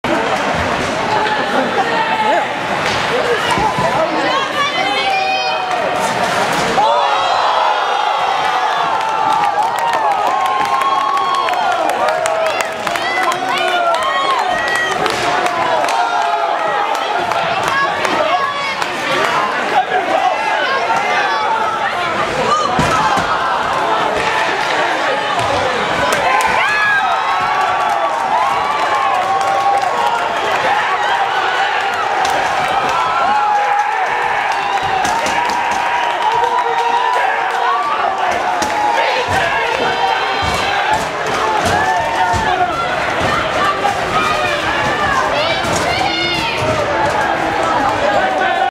Twin Valley Mifflin Varsity Hockey Fight.mpg
The video starts with Lew's hit and then Ian, Kevin, and Smitty's line rush.  Smitty gets the goal and then gets railed by #4.  Ian comes to his defense, then Kevin, Lew, and Kyle join the party.  This was just part of the beatdown.  More videos to follow...